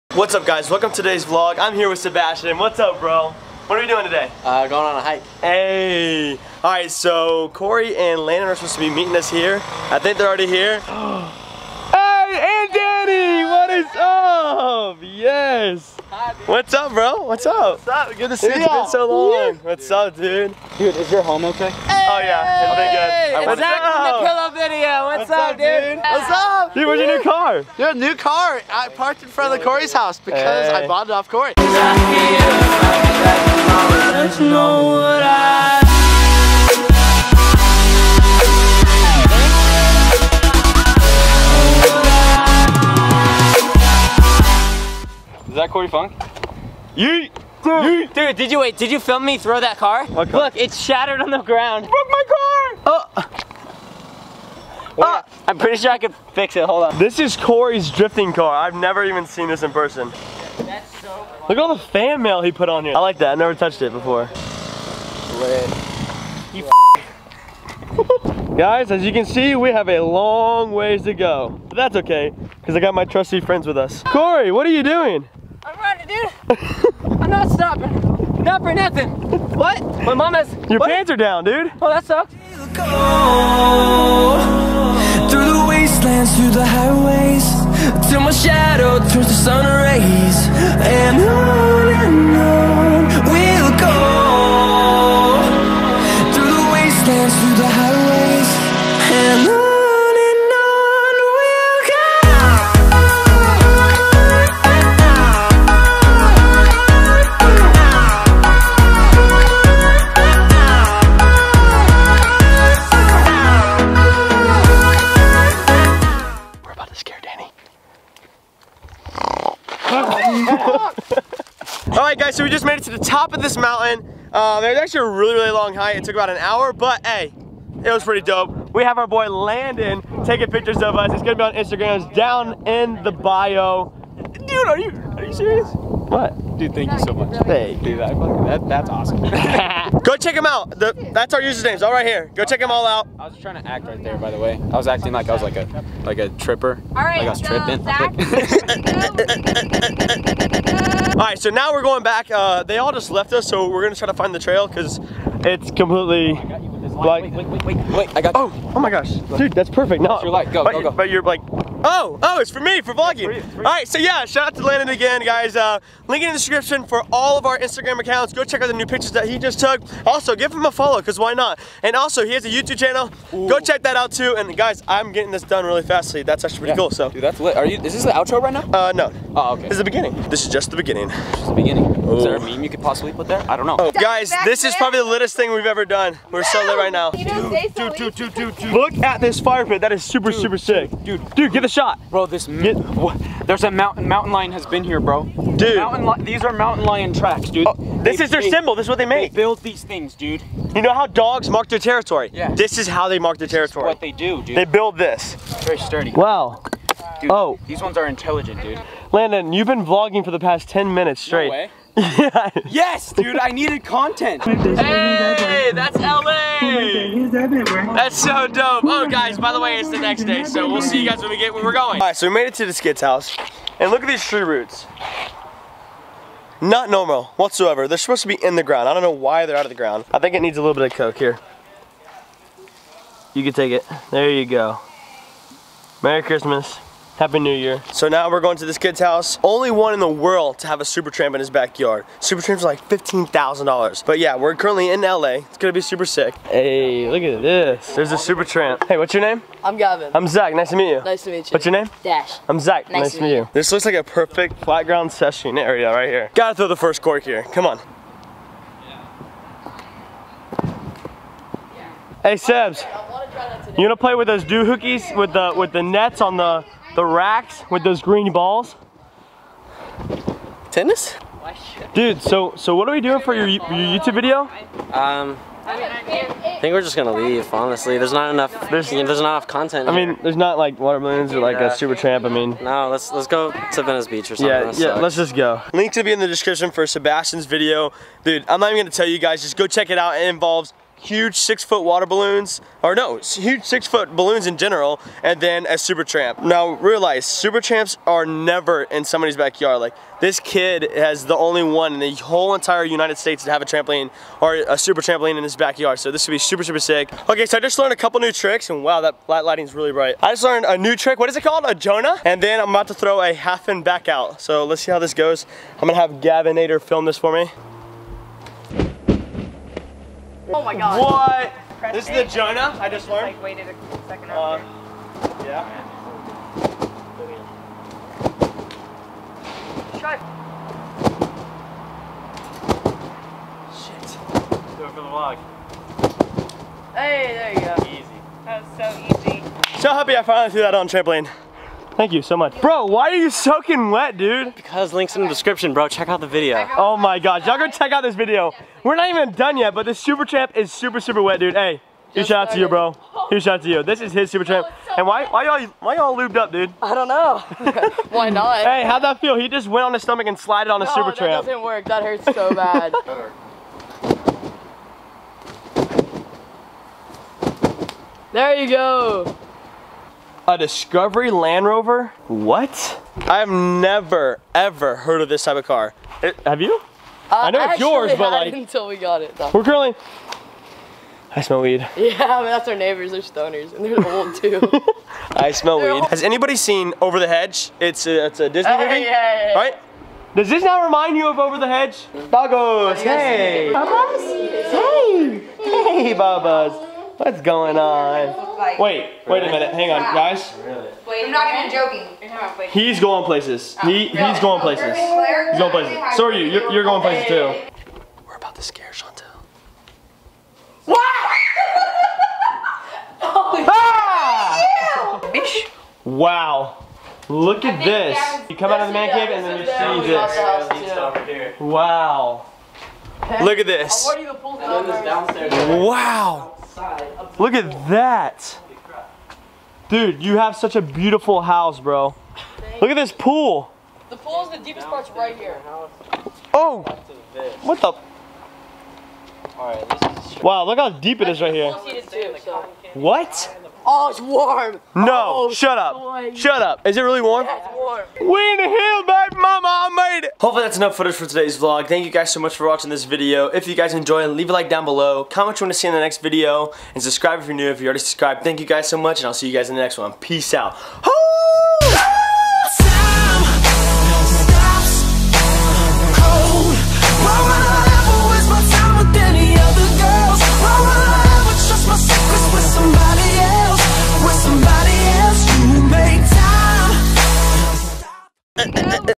GOT LOST WHILE HIKING!!
Anyways this was my first hiking experience with the crew! we saw a mountain lion off camera but saw a coyote also! pretty crazy day! ALSO went to a kids house in la who has the only super tramp in the entire world!

If your reading this comment "Boonk"